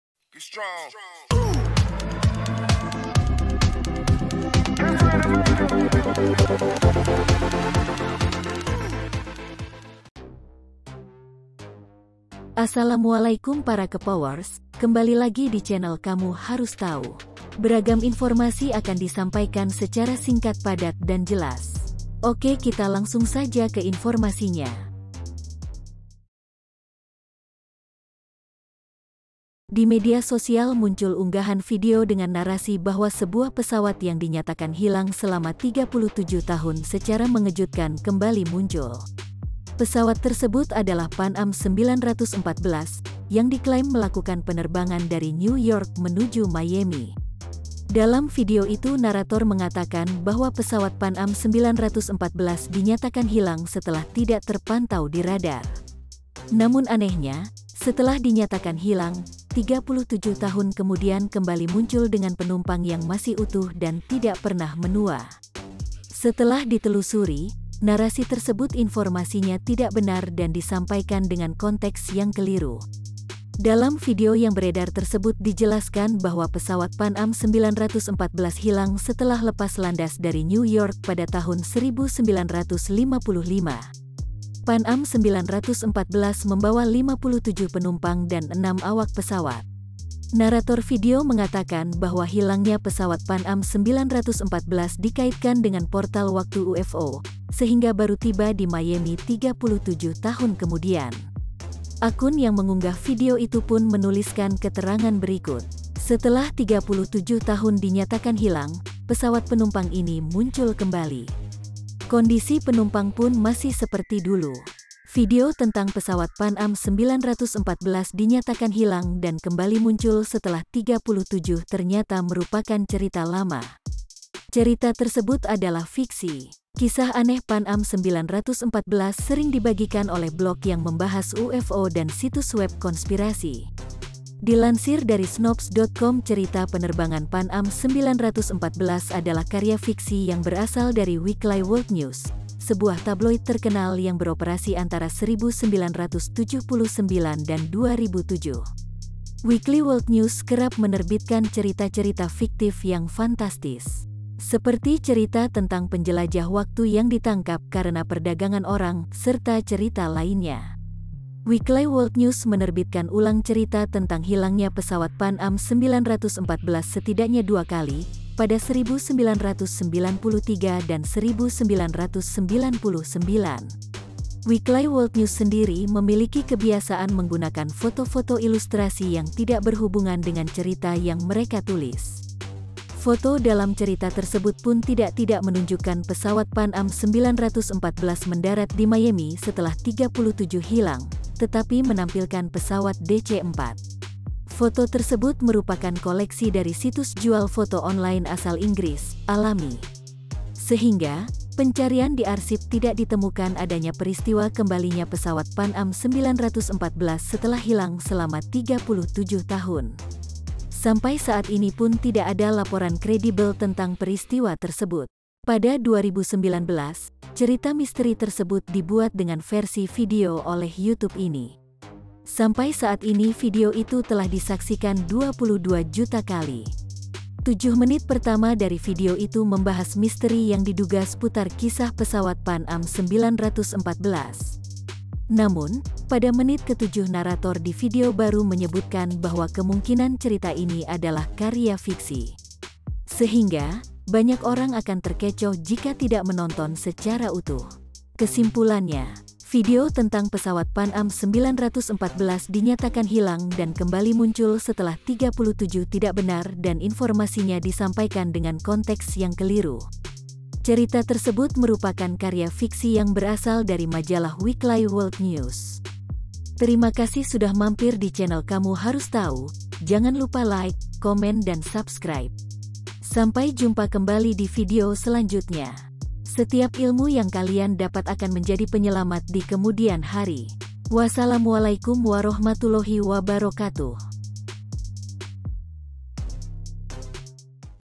HOAX !! Cerita Pesawat PAN AM 914 Hilang Selama 37 Tahun Kembali Muncul
Hello Kepowers kali ini mimin Mau share info mengenai KLARIFIKASI !! Cerita Pesawat PAN AM 914 Hilang Selama 37 Tahun Kembali Muncul . yukk tonton videonya sampai akhir.

Pesawat tersebut adalah PAN AM 914, yang diklaim melakukan penerbangan dari New York menuju Miami. Dalam video itu narator mengatakan bahwa pesawat PAN AM 914 dinyatakan hilang setelah tidak terpantau di radar. Namun anehnya, setelah dinyatakan hilang, 37 tahun kemudian kembali muncul dengan penumpang yang masih utuh dan tidak pernah menua. Setelah ditelusuri, narasi tersebut informasinya tidak benar dan disampaikan dengan konteks yang keliru.